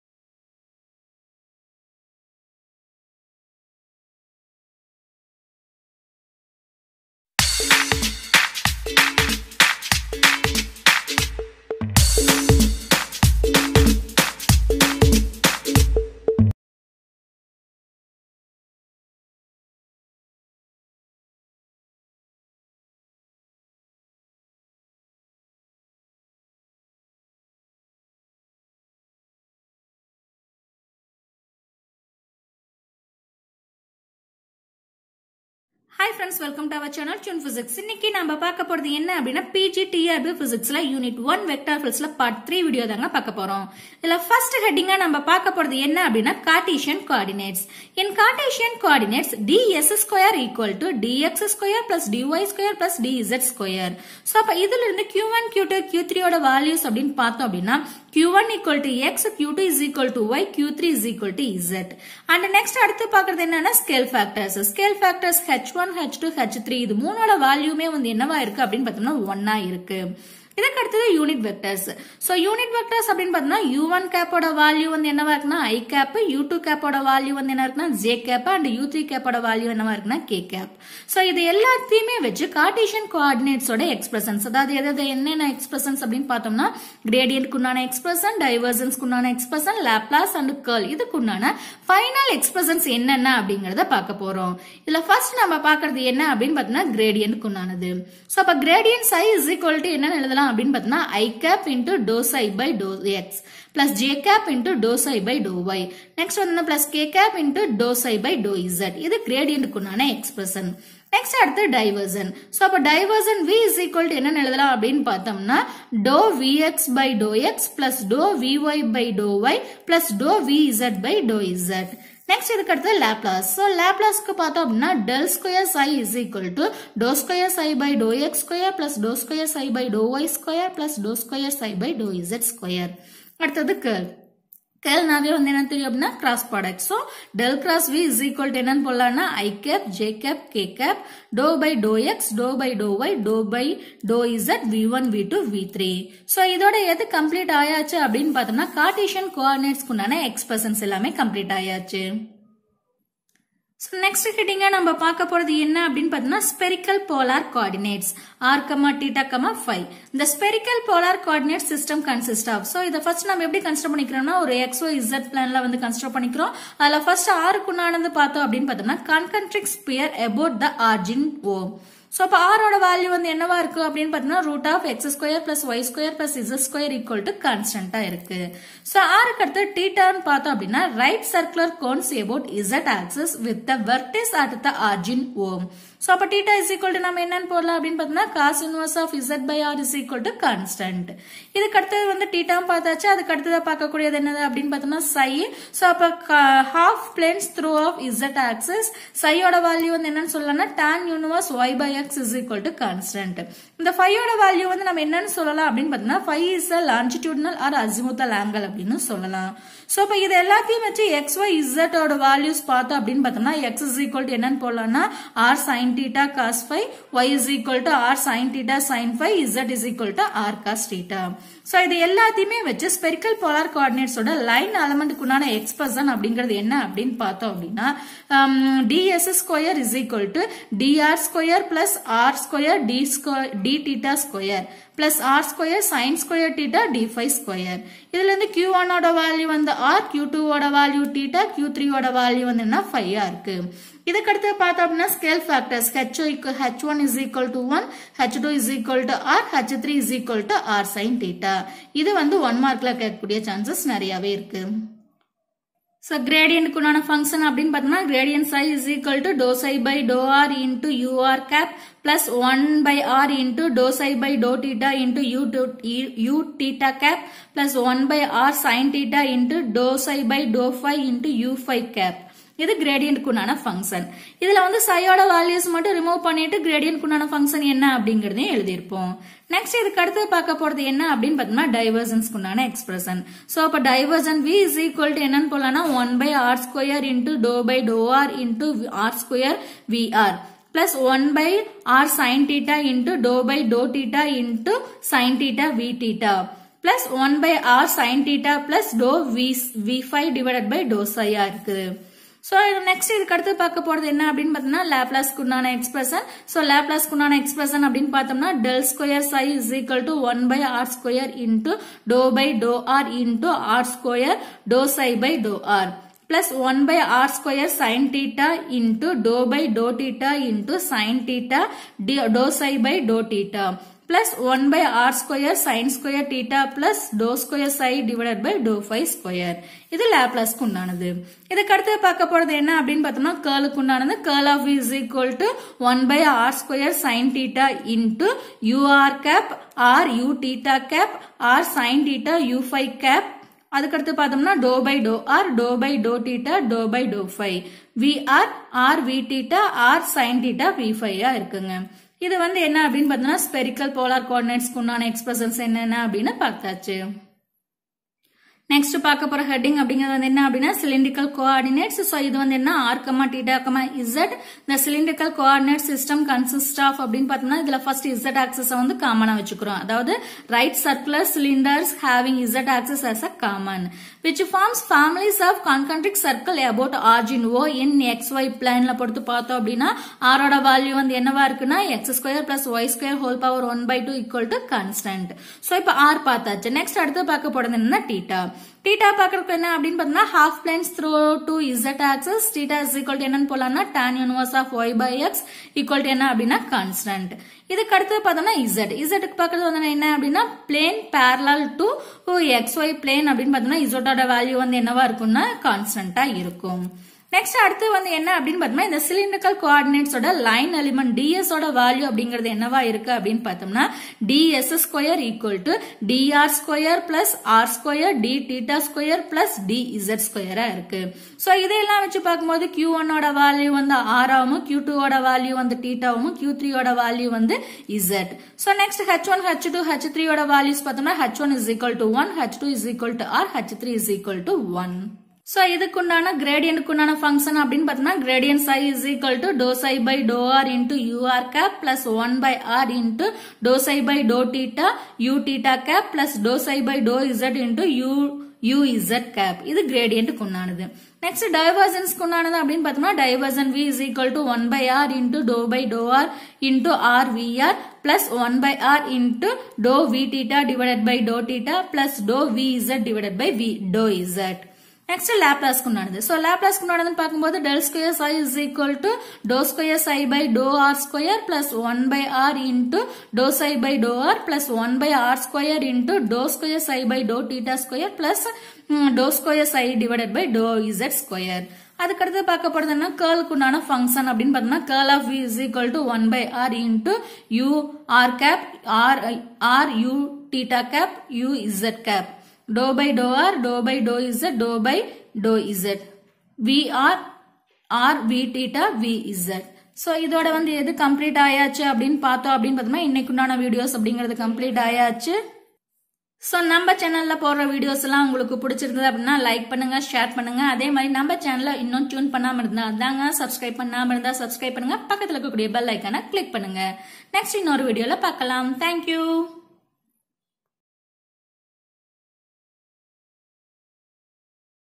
PG TRB Physics notes 📝 /1.Vector Fields Part 3(Cartesian, Spherical polar, Cylinder Co ordinates).
You can watch this video PG TRB Physics notes 📰

Unit : 1.Vector Fields Part 3

Topic : Cartesian Co ordinates, Spherical polar Co ordinates, Cylindrical Co ordinates.

🎶 PG TRB PHYSICS Study materials available 🎶

🎶 TRB Polytechnic Physics materials available 🎶